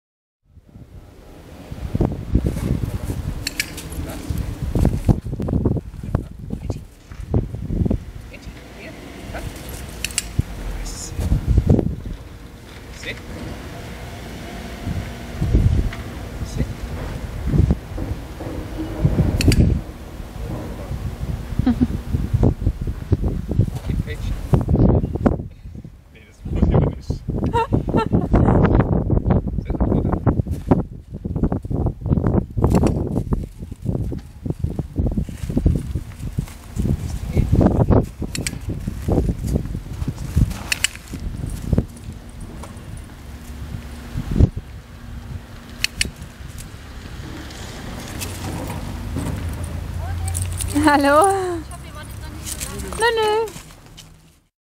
Hopp und Warten auf Ronja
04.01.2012 - Wir treffen uns mit Ronja. Am Anfang üben wir mit Angie hoch und runter auf den Stein, was Angie sehr gern macht. Als Ronja dann kommt, zeigt Angie sie an. Sie kommt jedoch viel zu schnell auf uns zu, weshalb Angie überfordert ist und am Ende auch in die Leine springt.
Normalerweise üben wir mit ihr "Zeigen & Benennen", was in diesem Fall nur bedingt ging.